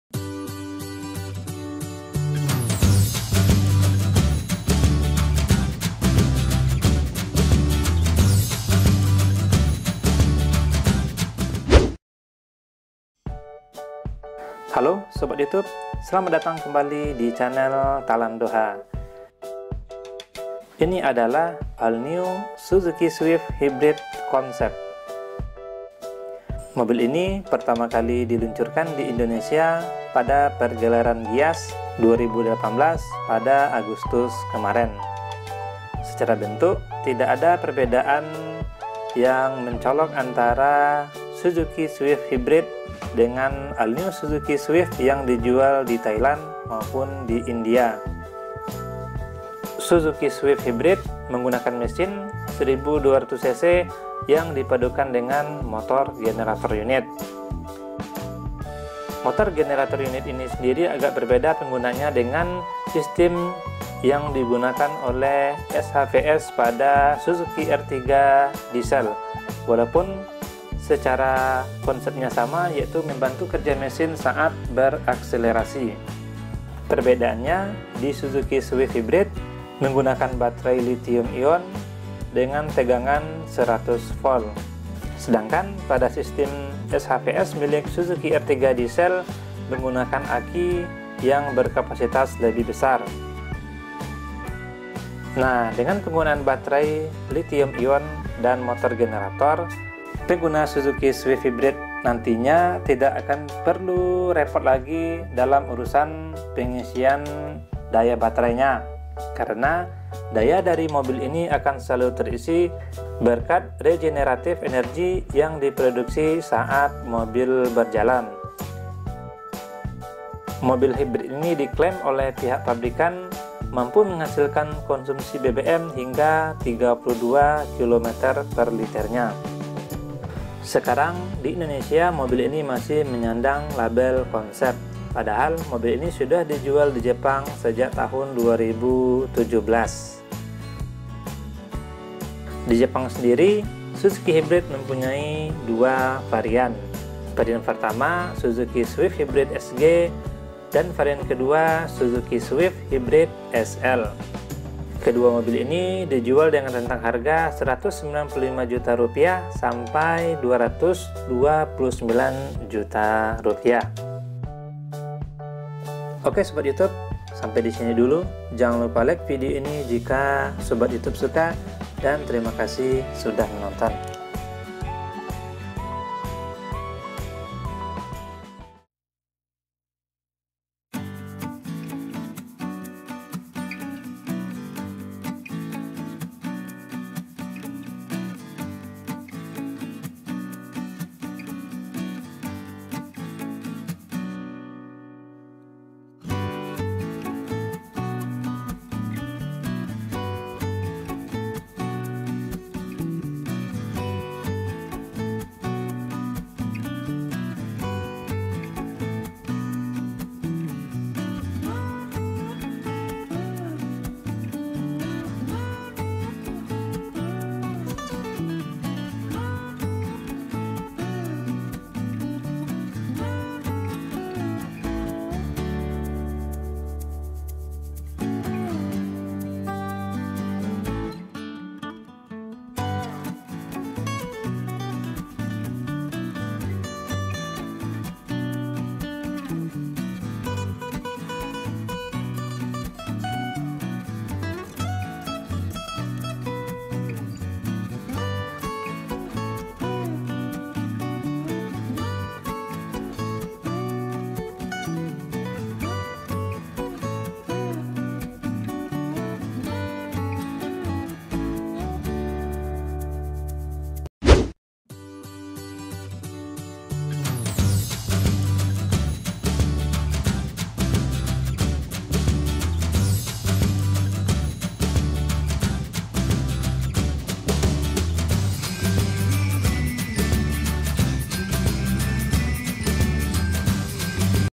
Suzuki Swift Hybrid 2018 Indonesia Pesaing Honda Jazz dan Toyota Yaris
Diperkenalkan Di Indonesia Pada GIIAS 2018, Suzuki Swift Hybrid 2018 Menggunakan Mesin 1200cc Dengan Bantuan Motor Generator Unit Dan Battery Litiom ion 100 Volt